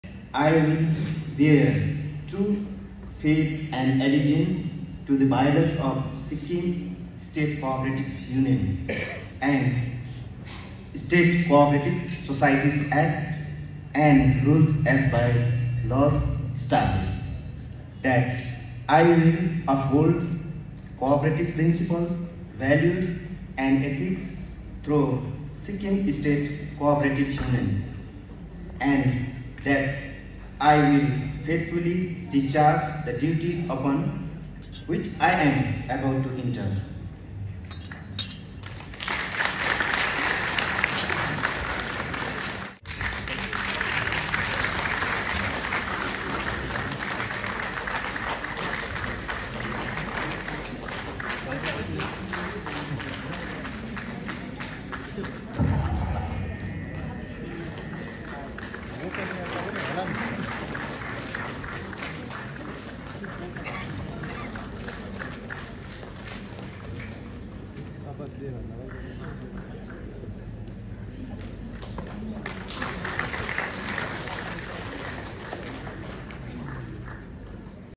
Mangaljit Rai takes the oath as Chairman of SICUN
Mangaljit Rai takes the oath as Chairman of SICUN in the presence of Arun kr upreti, Minister of RDD and Cooperation department government of sikkim